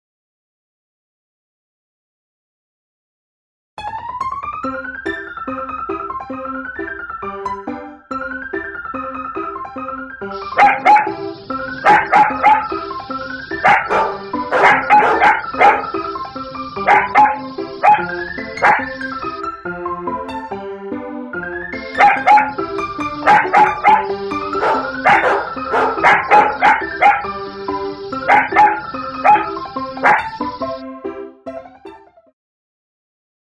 The Big Chase
Seaweed meets a couple of little doggies at Ponderlodge—Billy and Rocky—and one of them gives him a run for his money.  Guess what?  There is nothing Seaweed likes more than being chased—he feels invincible!